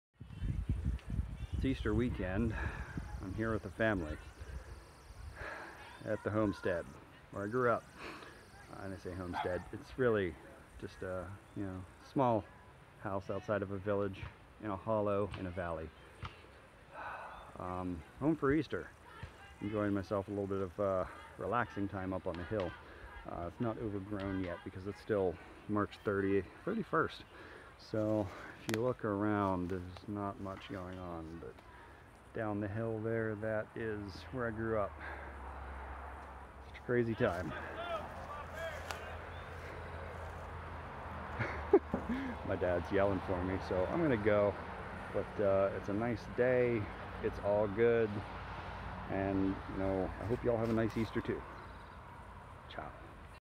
Hometown Hillside Easter
I almost got run down by a crazed old dude driving a quad when I was 15 climbing up this hill.  No wonder I stopped going outside.